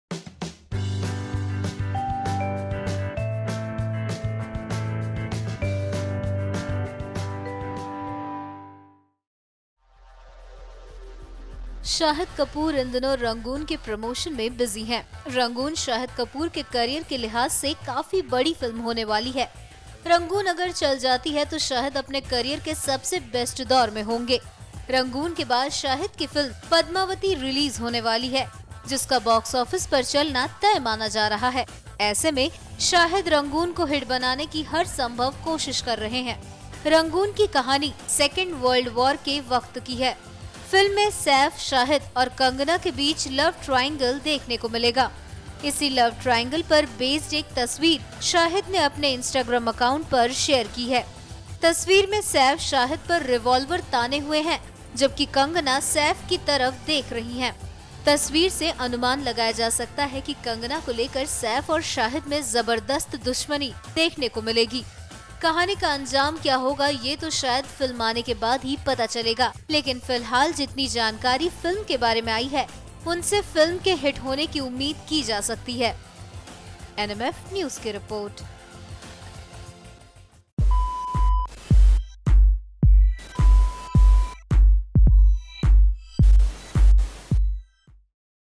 Shahid Kapoor ने Share की Rangoon की नई Photo
Bollywood Star Shahid Kapoor is Nowadays busy in promoting his upcoming Film ‘Rangoon’ . He shared one of his recent pic from the sets of Rangoon on Instagram where all the leading actors are captured in one frame .
NMF: शाहिद कपूर ने रंगून की नई तस्वीर शेयर की है. तस्वीर में सैफ शाहिद पर बंदूक ताने खड़े हैं.. ये सीन फिल्म का आखिरी सीन होने का अनुमान है